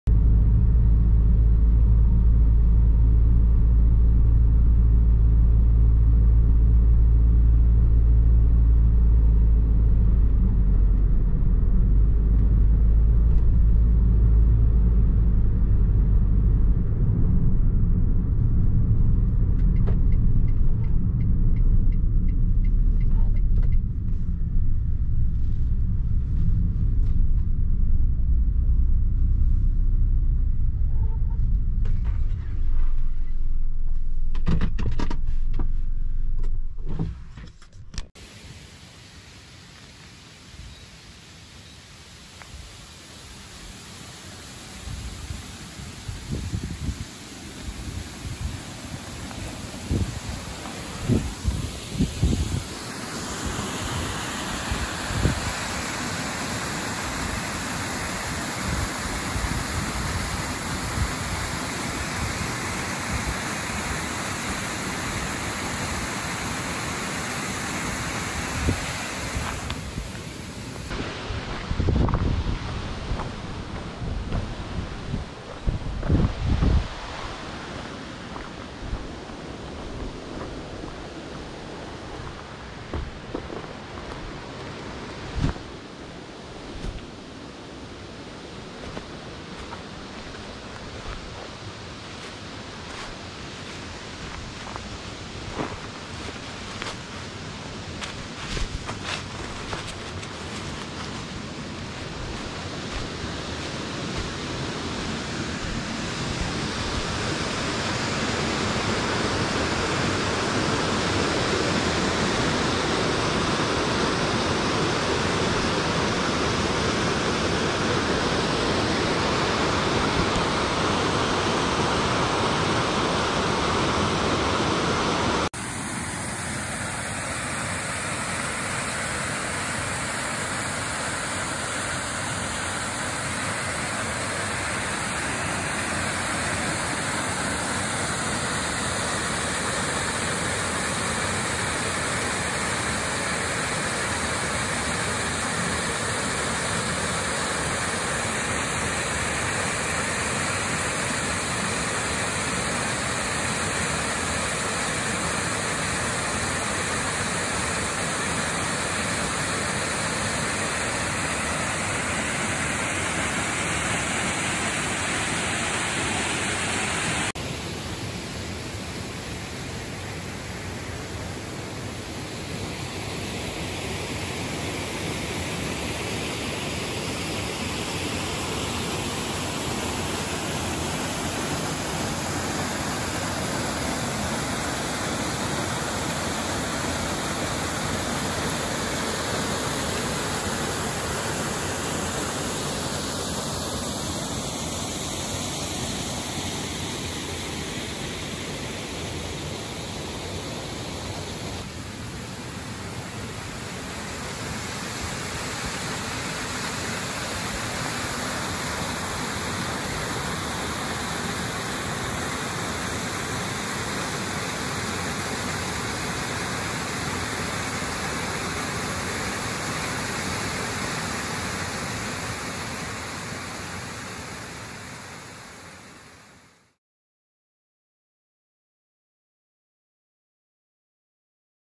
（栃木名瀑巡り）栃木県道277号小来川清滝線 大滝
黒川、大滝。

このエリア、大滝と名のつく滝がやたらと多くて、困惑。
道路わきの場所から滝の目の前まで歩いて近づける小道があります。
足場要注意ですが、駐車スペースからすぐ覗きこめるお手軽な滝です。